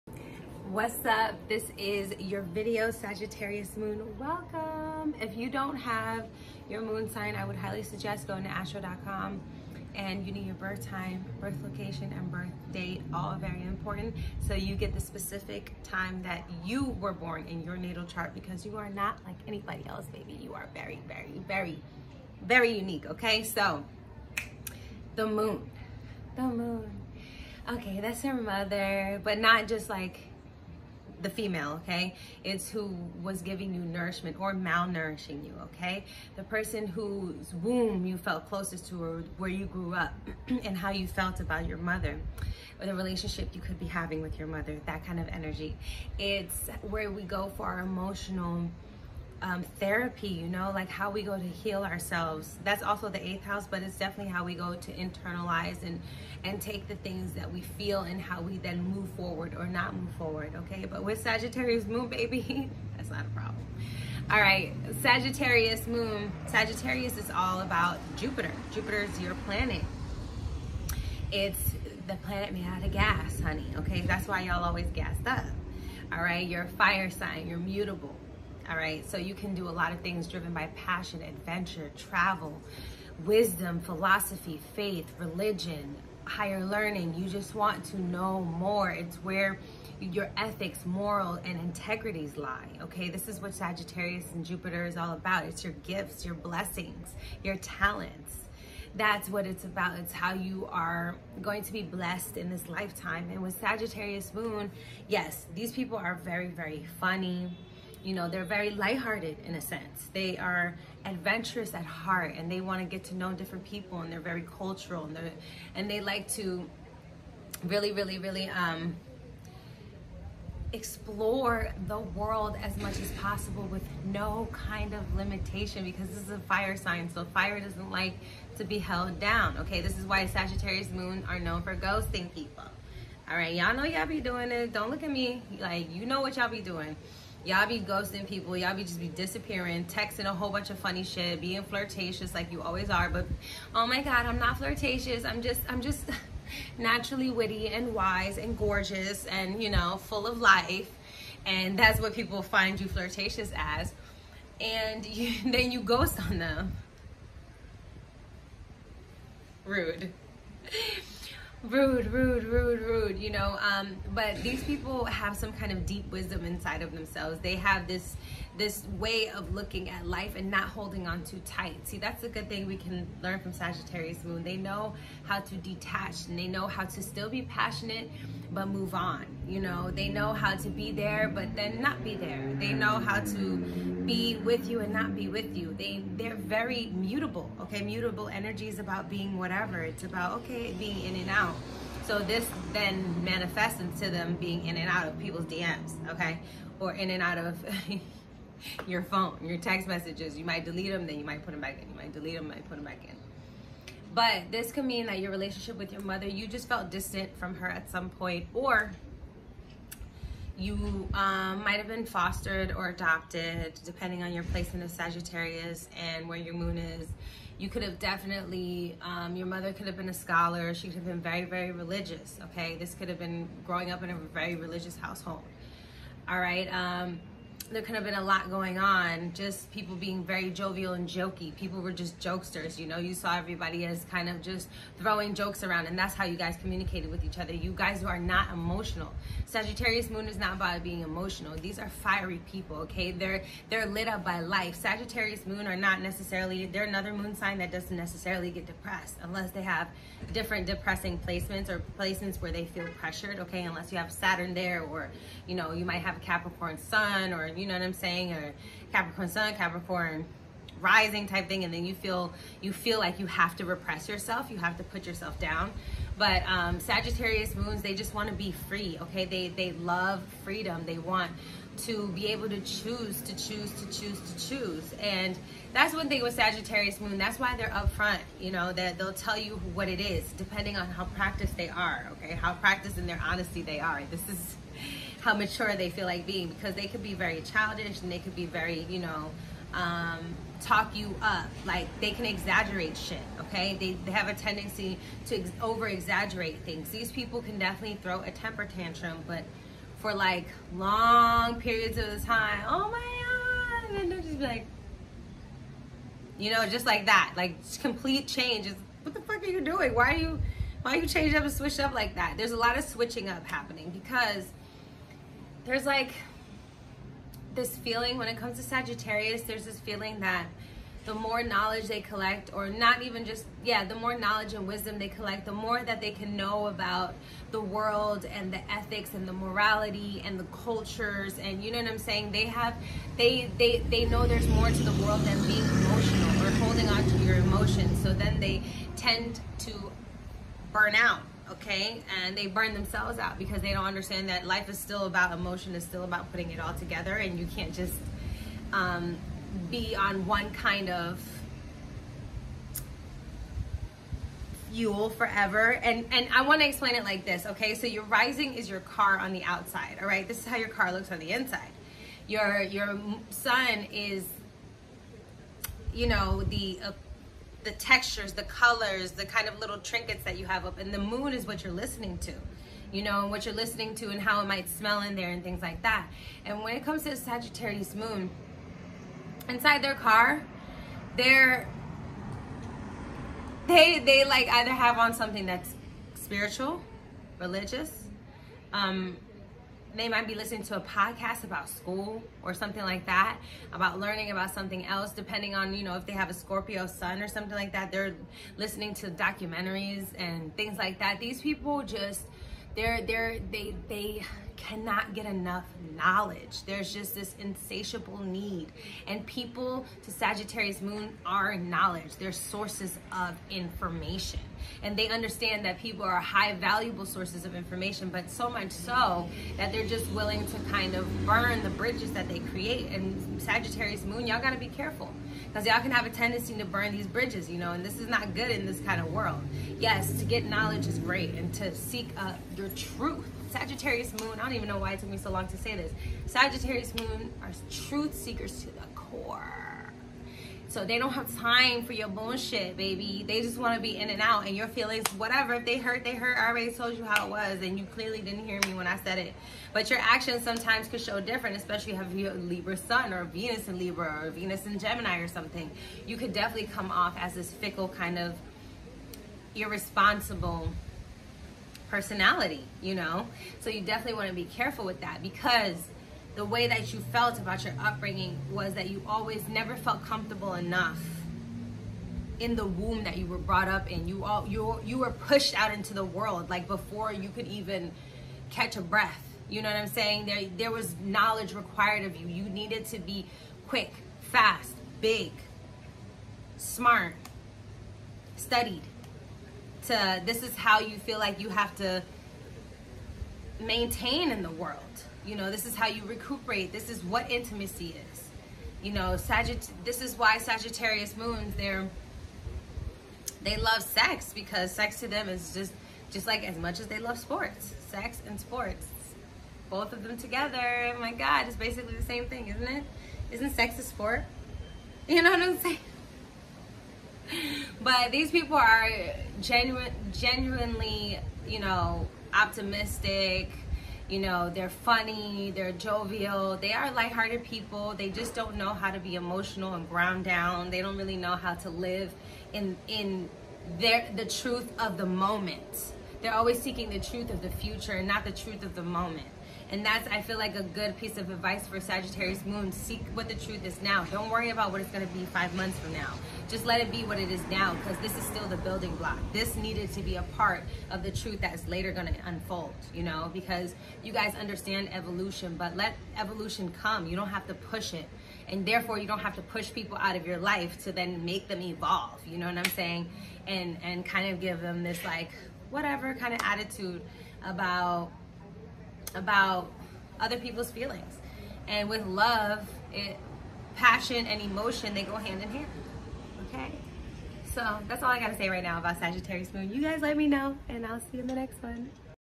The Life Of A SAGITTARIUS MOON ♐️ 🌚 •••
Hey family!

Just a quick insight on Sagittarius. Signs and aspects matter. To get a further look into your Sagittarius moon, get a natal chart. This is a GENERAL reading and doesn’t apply to all people. There is an entire story to your chart. 🤎

You can use Astro.com to find all your placements.